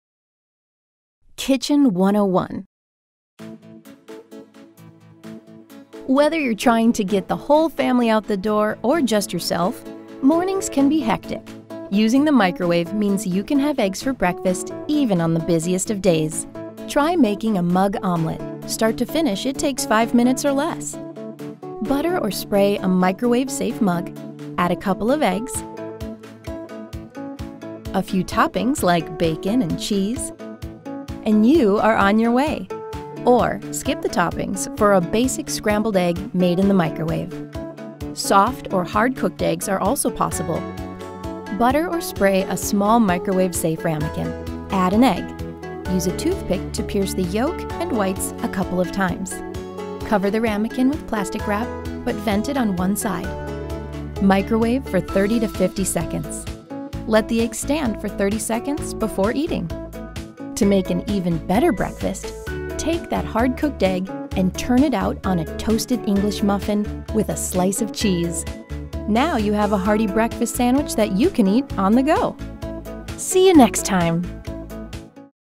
Microwave Eggs | DIY & How To | Kroger
Eggs can be cooked easily in the microwave, learn how in this video.